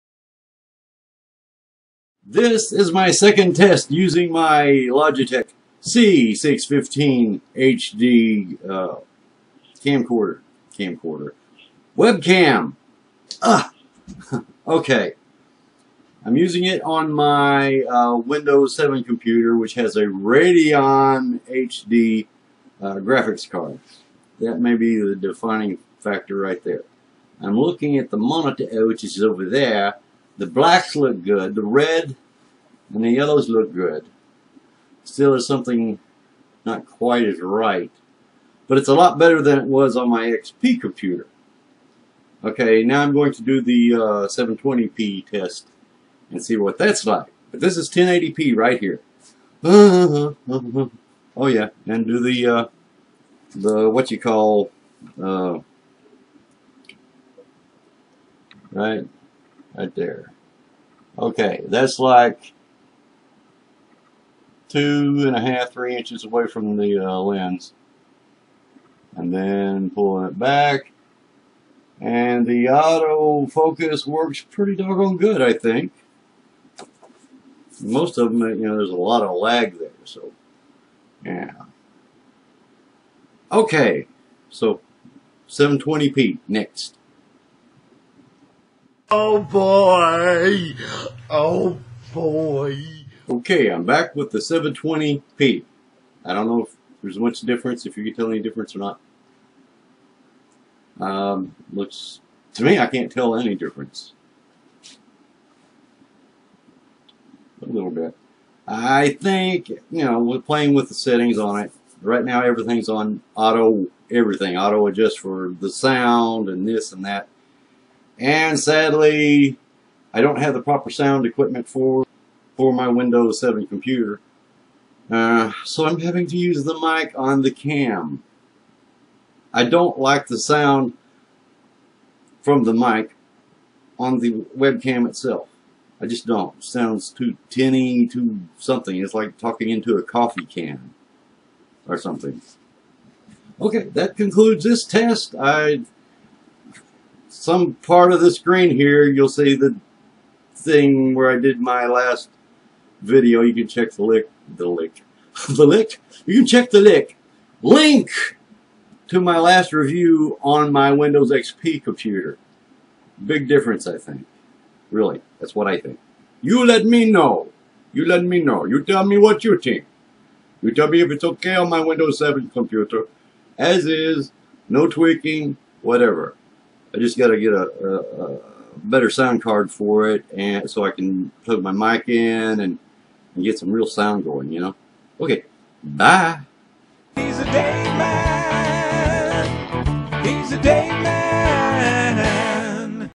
LOGITECH C615 HD Test No.2 on Windows 7
Testing the Logitech C615 HD 1080p webcam on my Windows 7 computer, since it did not perform well on my Windows XP system. To see the results on my Windows XP computer, go
TESTING ON: Windows 7 - AMD Athlon II 3.0Ghz - 2GBs DDR2 Ram - Radeon HD 4350 graphics card